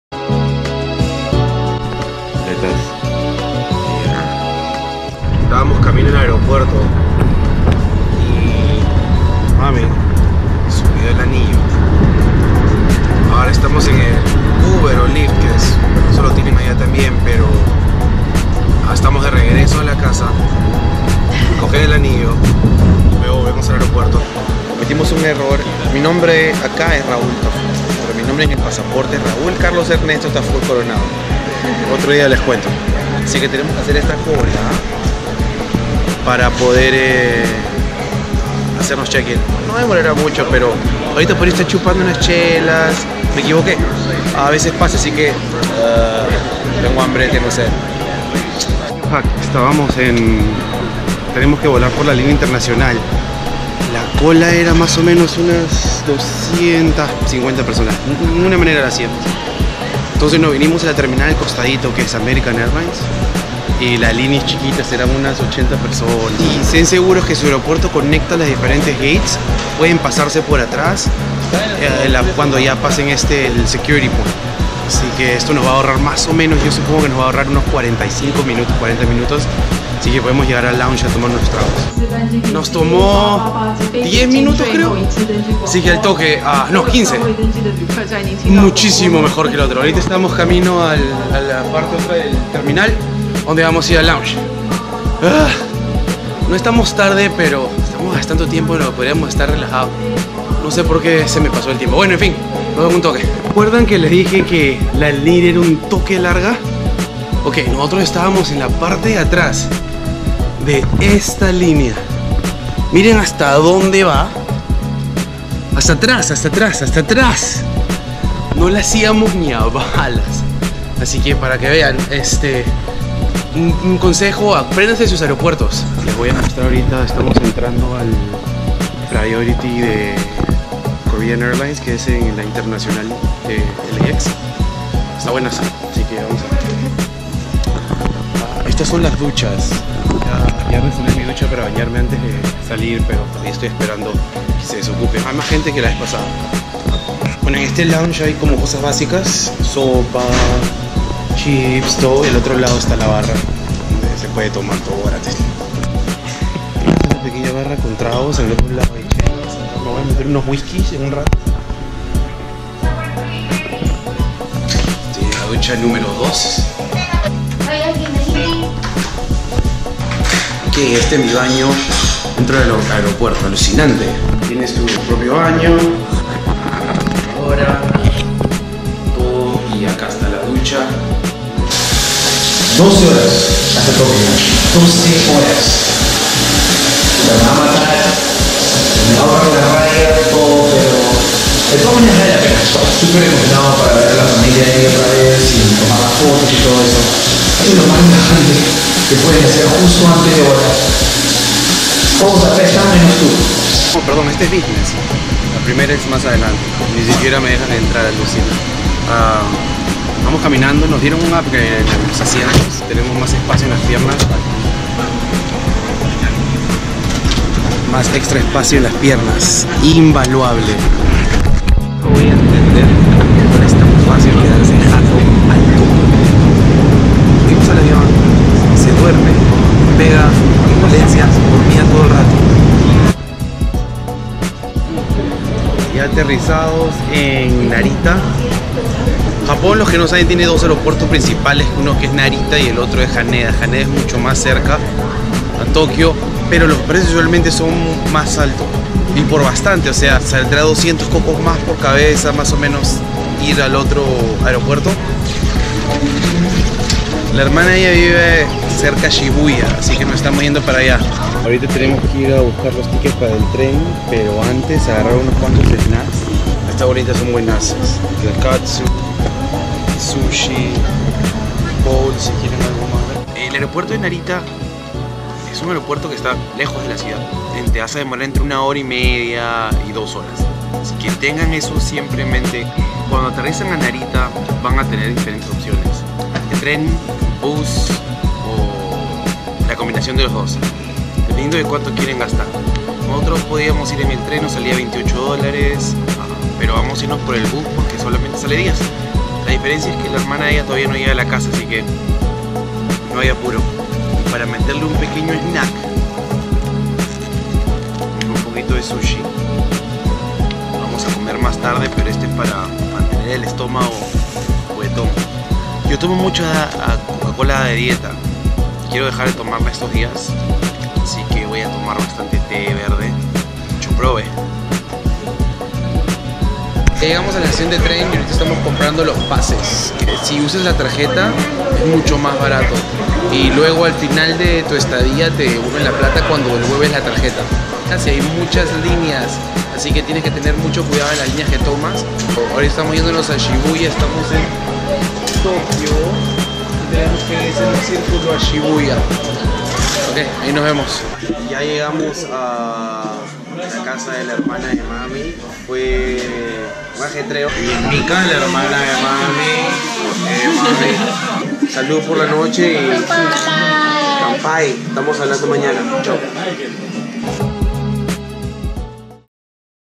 Vlog 02 - De Los Angeles a Japon LAX-NRT l Traveling to Japan for cheap
Finalmente llegamos al aeropuerto pero con un poquito de demora. Vip lounge, Colas y todo el ajetreo que trae viajar.